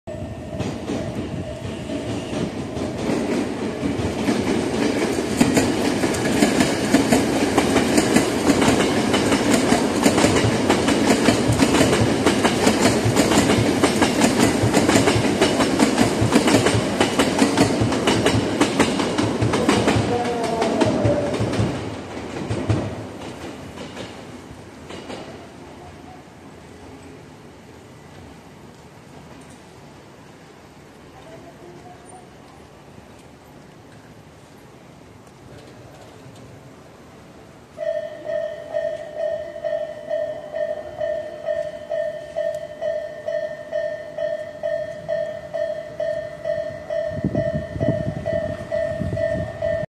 🔶※ﾒﾄﾛ03系 3ﾄﾞｱ｢北越谷｣LED編成
撮影日:20170509
撮影地:東武ｽｶｲﾂﾘｰﾗｲﾝ竹ノ塚→谷塚
🔶=下り撮影
※将来、下り仮ﾎｰﾑ移設(西側)